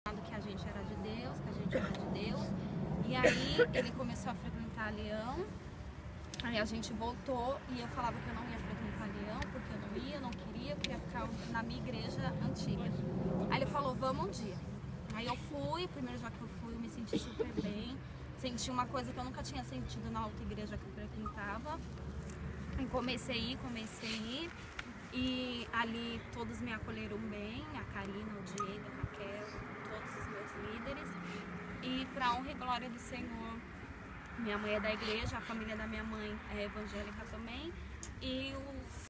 Pastor Thiago Barbosa
4º Parte dos testemunhos dos batizados da Leao da Tribo de Juda em 05/09/2015